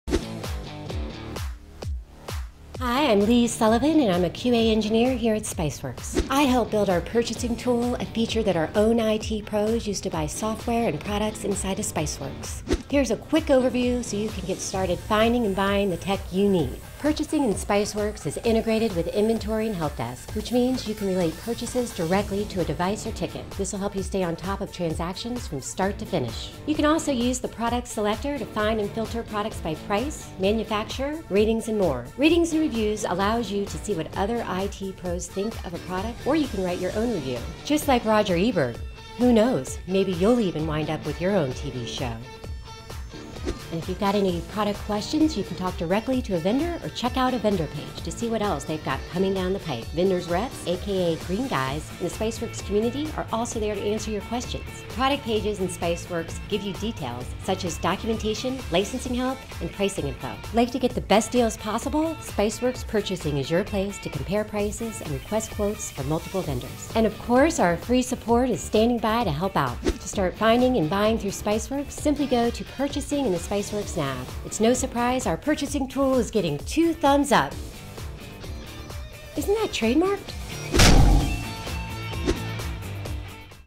Research & buy tech in Spiceworks
From printer ink to cloud servers, buy everything IT directly in Spiceworks! Get great deals from tech vendors thanks to the collective buying power of Spiceworks.

-Associate purchases with devices and tickets
-Request quotes from multiple vendors
-Research and compare prices across devices and vendors
-Let your voice be heard by rating and reviewing products

Check out this quick vid as Spiceworks developer Leigh S. shows you how to make purchasing a breeze in Spiceworks.